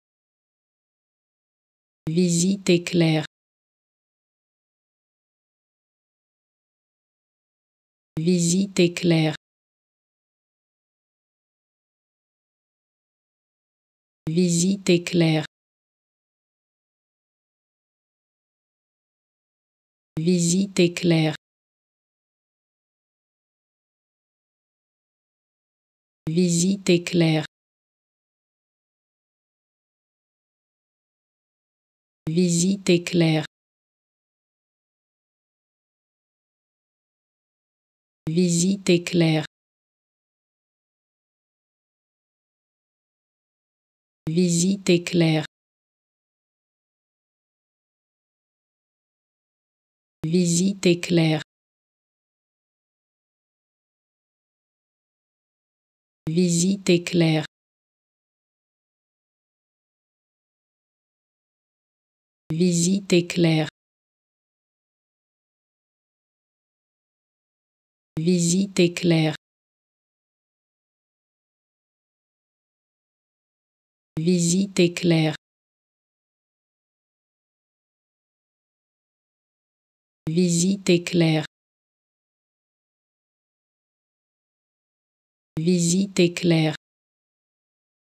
How to Pronounce ''Visite-éclair'' (Quick tour) Correctly in French
Learn how to say and properly pronounce ''Visite-éclair'' in French with this free pronunciation tutorial.